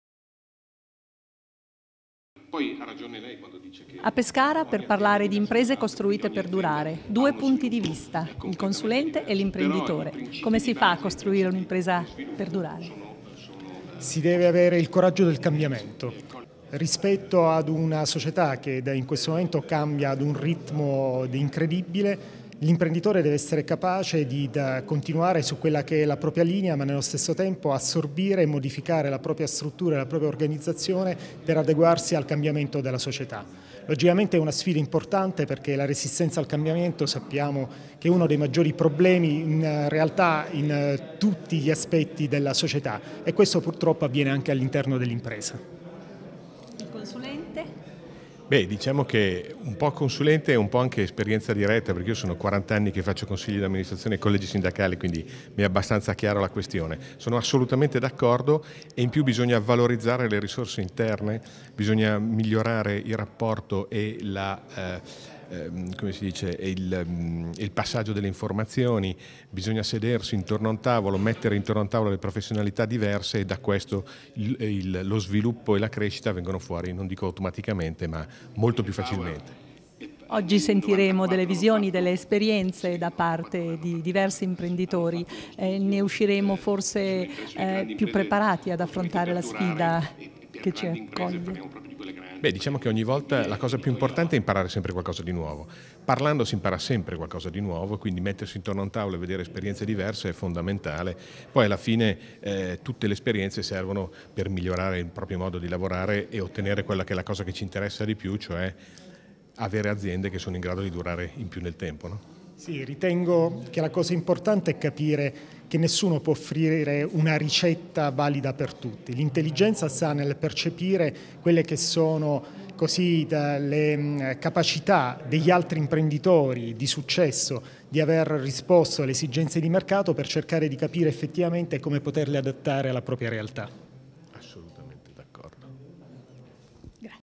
Enrico Marramiero - Alma CIS + Enrico Maria Bignami +Bignami Associati
Intervista a Enrico Marramiero e Enrico Maria Bignami al Workshop "Imprese costruite per durare" organizzato da Bignami Associati , con l'Azienda Vinicola Marramiero e la Dike Consulting, presso l'Azienda Vinicola Marramiero di Rosciano (PE) il 14 novembre 2014